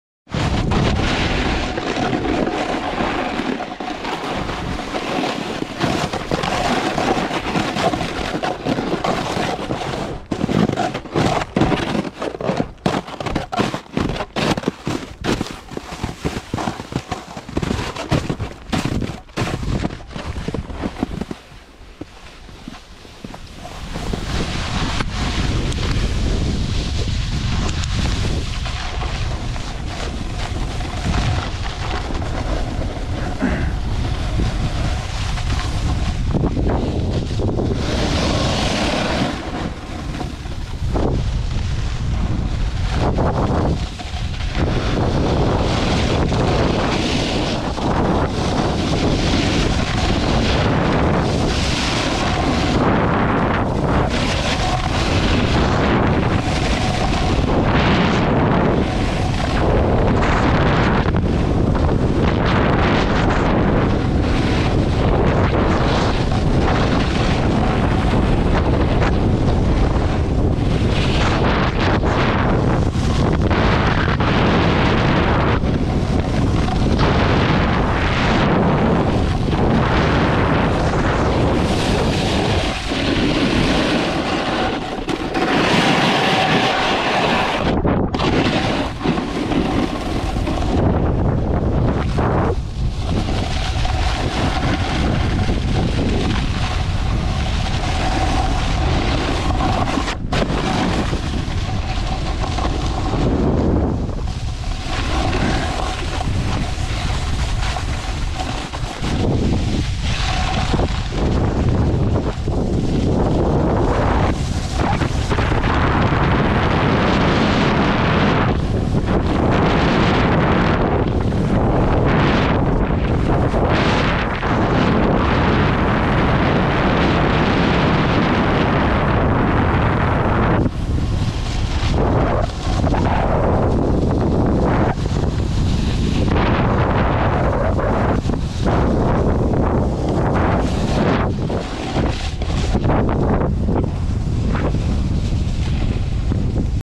Snowboarding sierra at tahoe 3/29/2021
snowboard are my thing, and this is how i use them enjoy